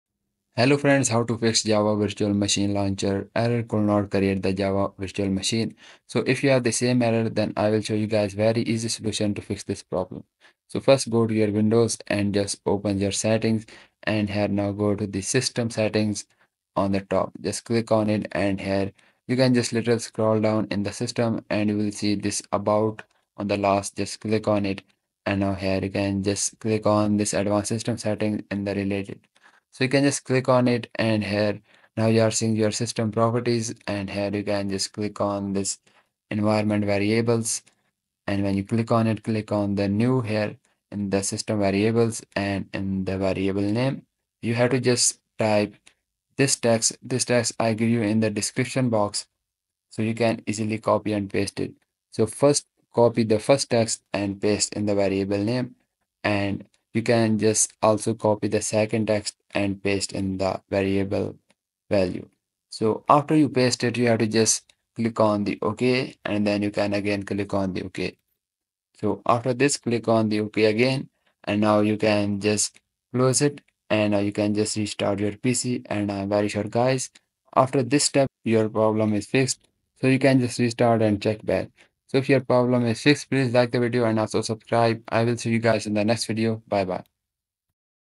[FIX] How to Fix Could not create the java virtual machine | How to Fix Fatal Exception has Occured
Error: Could not create the java virtual machine | How to Fix Fatal Exception has Occured

Commands:

_JAVA_OPTIONS

-Xmx512M

Are you struggling with the frustrating error "Could not create the Java virtual machine"? This informative tutorial video has got you covered! Discover easy-to-follow steps and expert tips on how to troubleshoot and fix this common issue. Say goodbye to endless search results and get your Java up and running in no time with this comprehensive guide. Don't let technical difficulties hold you back - hit play now and become a Java pro! Master the Java virtual machine with this must-watch video!